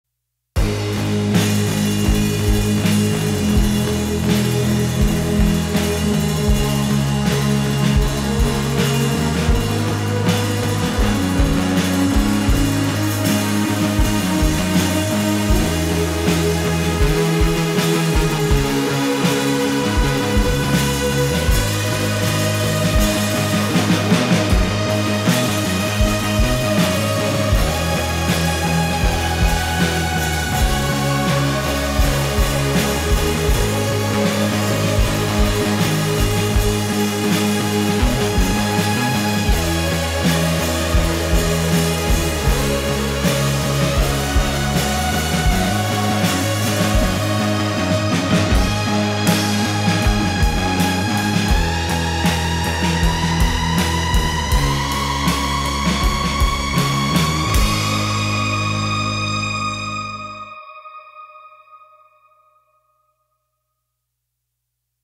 Jamuary 2021 - Day 3
jamuary2021 - Day 3
Short little synth rock song and my debut on drums 🥴